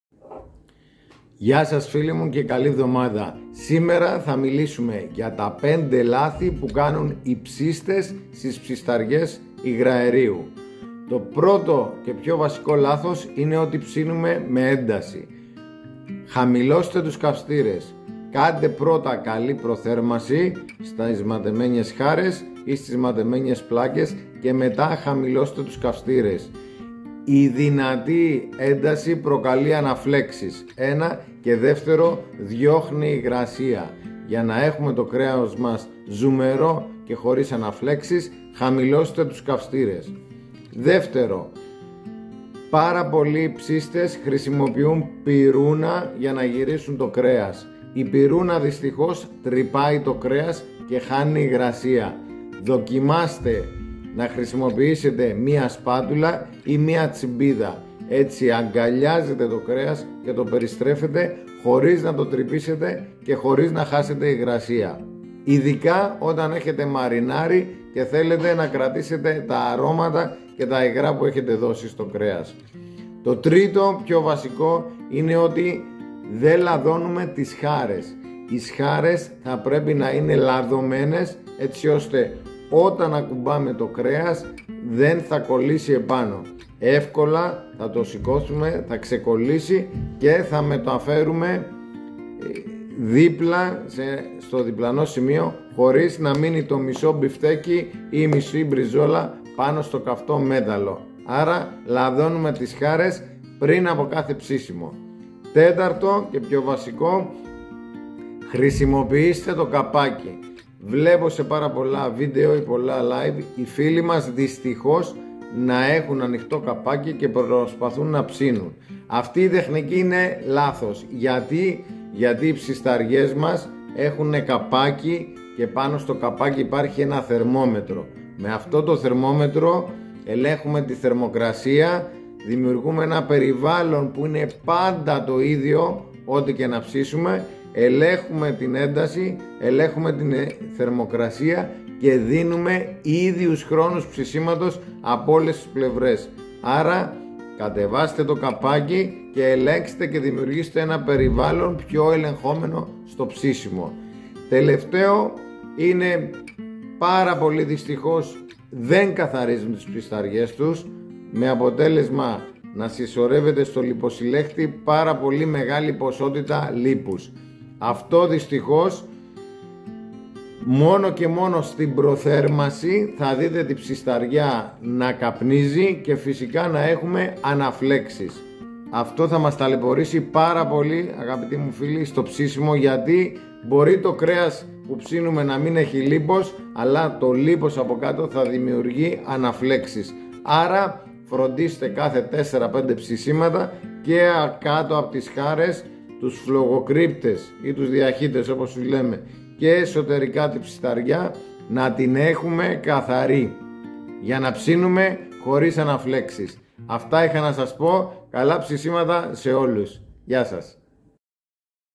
5 Βασικά ΛΑΘΗ στο Ψήσιμο στο Υγραέριο
Συμβουλές για ζουμερό ψήσιμο χωρίς αναφλεξεις 🔥